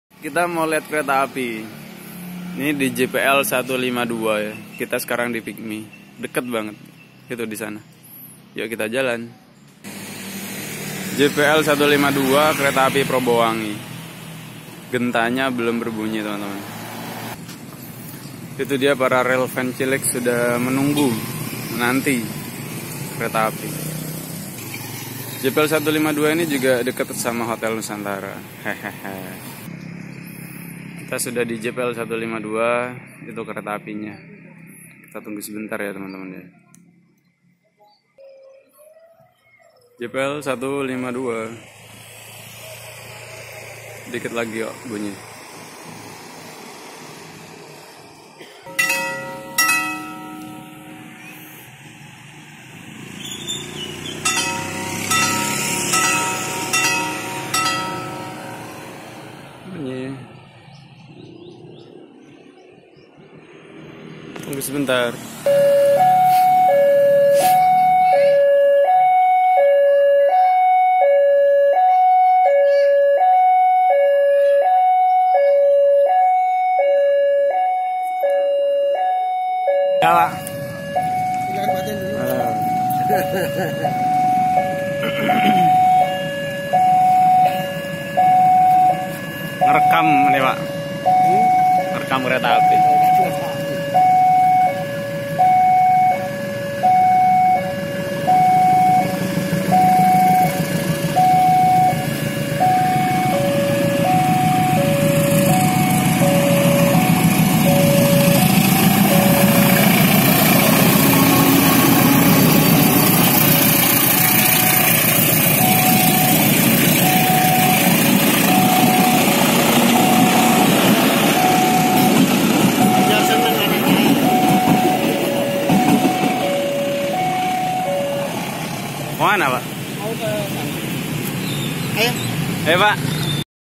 Lihat Kereta Api Tawangalun Jurusan Jember - Malang
Lihat Kereta Api Tawangalun Jurusan Jember - Malang : Lagi ngajari adik saya mamat untuk menjadi blogger ini, dimulai dari video kereta api ini ya. Semoga temen-temen semua disini bisa terhibur, amiin.